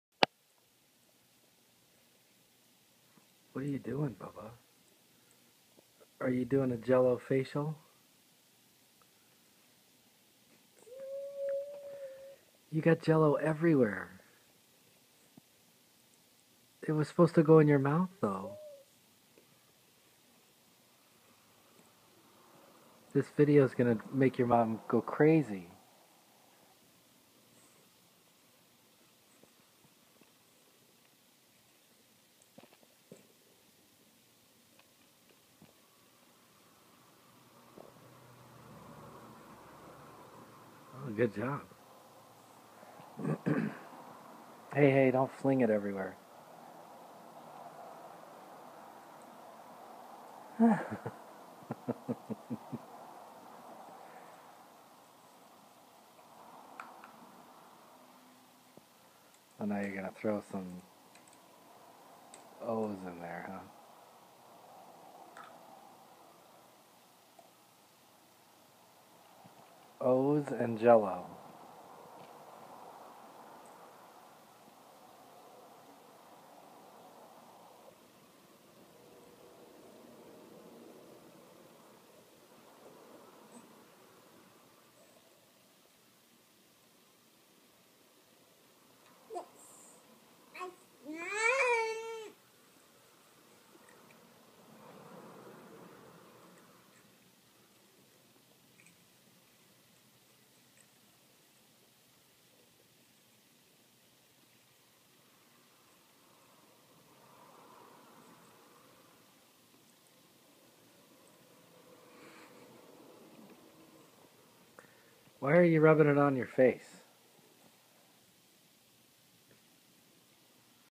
Jello facial
Ben rubs jello all a or himself